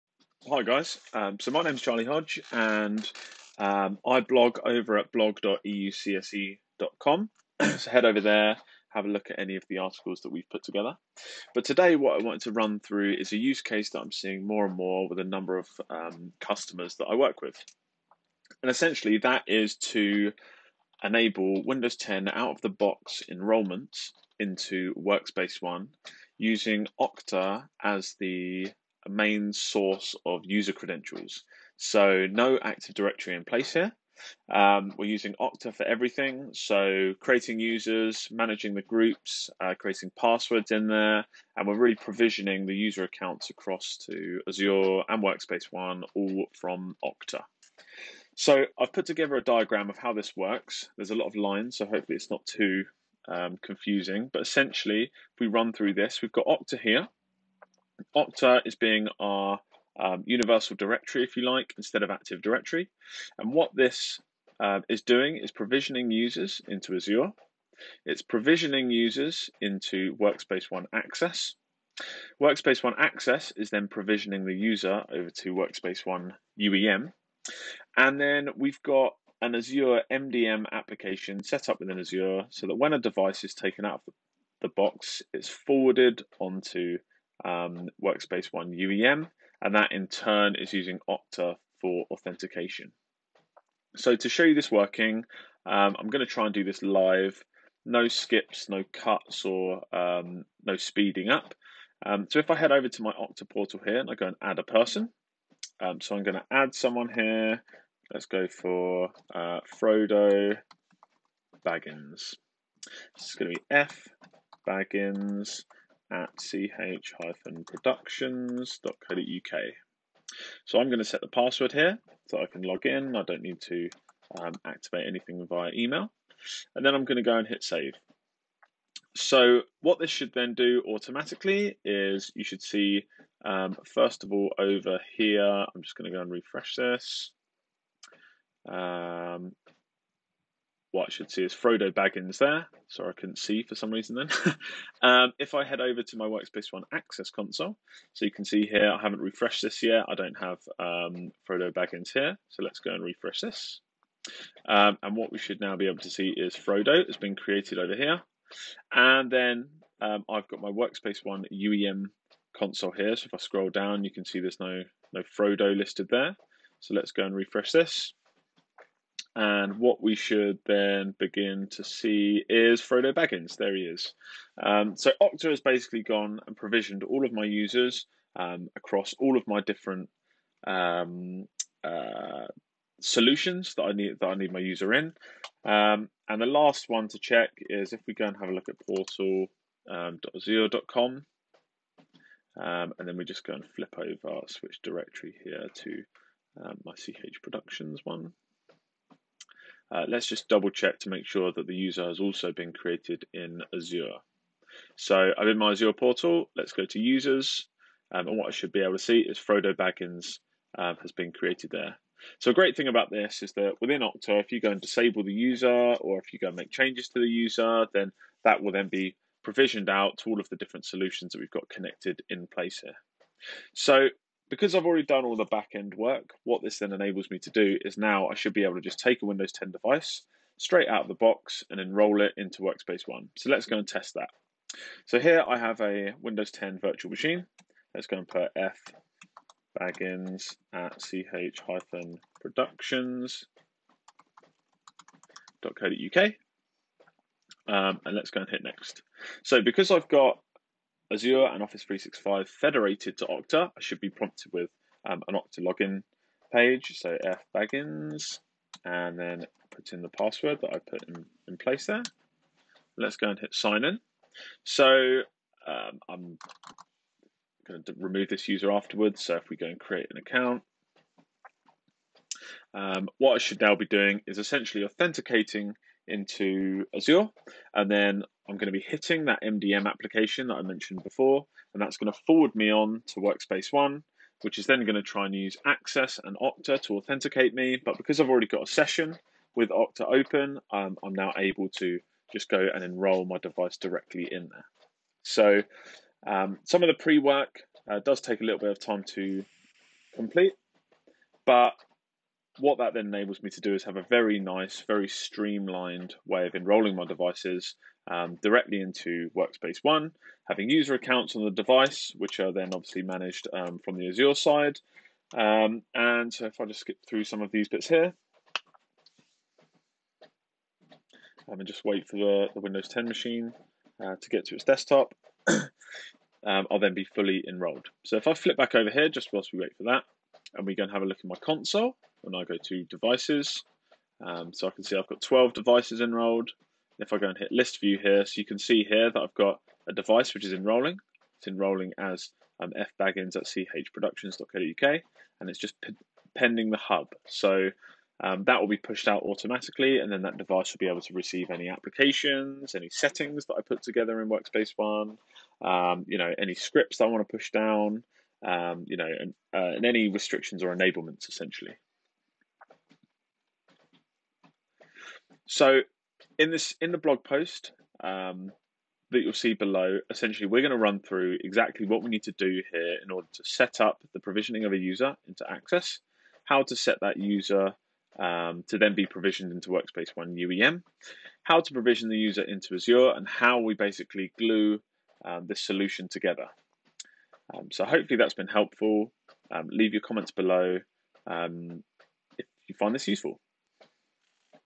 Windows 10 Azure Enrolment with Okta and Workspace ONE
Leveraging Okta to provision users into Azure and Workspace ONE to perform Windows 10 Azure based Enrolments into Workspace ONE.